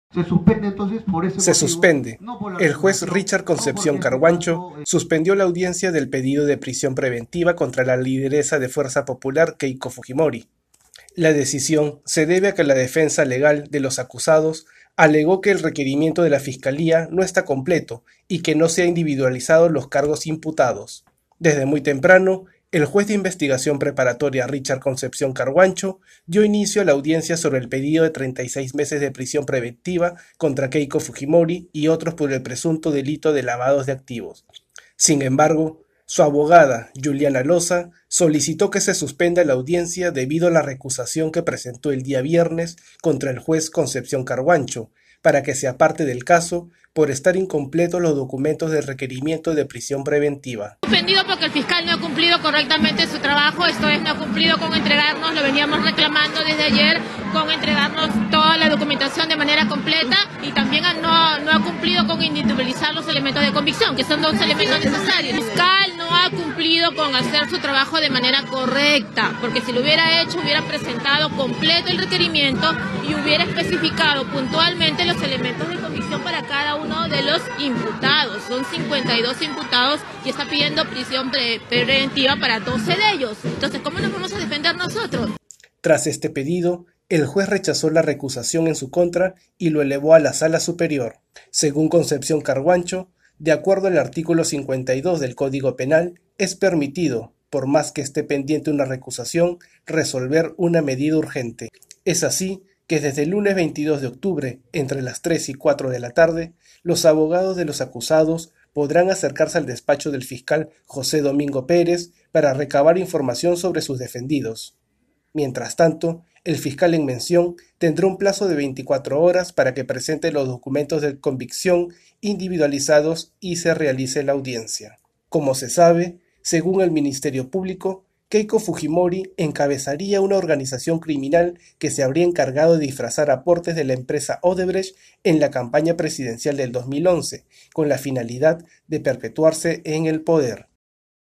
SE SUSPENDE AUDIENCIA DEL PEDIDO DE 36 MESES DE PRISIÓN PREVENTIVA CONTRA KEIKO FUJIMORI
EL JUEZ RICHARD CONCEPCIÓN CARHUANCHO SUSPENDIÓ LA AUDIENCIA DE PRISIÓN PREVENTIVA CONTRA KEIKO FUJIMORI Y OTROS 11 INVESTIGADOS PARA QUE LA FISCALÍA PUEDA COMPLETAR LOS EXPEDIENTES Y DOCUMENTACIÓN EN LA ACUSACIÓN FISCAL. LA DEFENSA DE LOS ACUSADOS HICIERON LA SOLICITUD DURANTE LA AUDIENCIA.